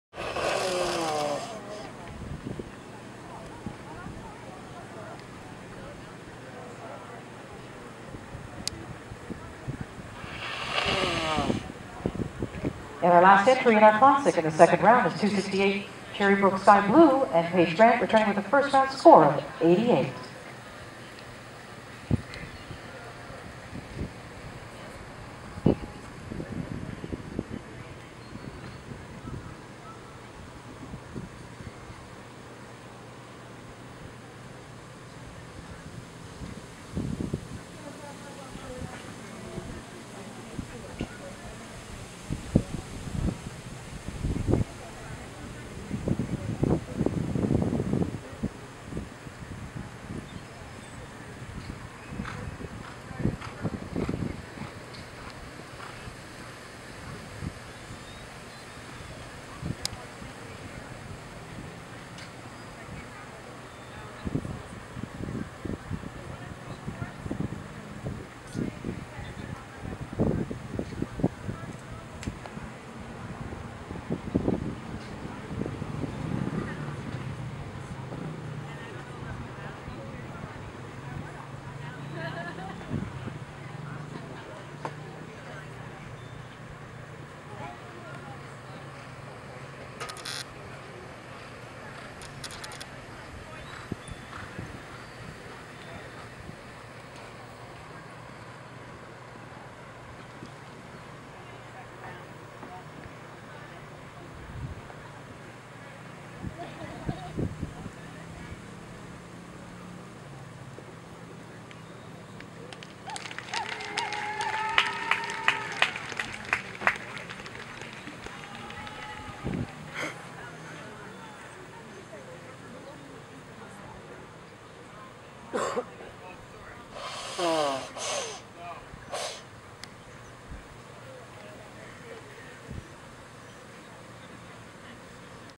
CherryBrook Skye Blue -Round 2
CherryBrook Skye Blue
Jump Canada Pony Hunter Classic
Large Pony Hunter

Round 2
Score: 87

Ridden By: Paige Grant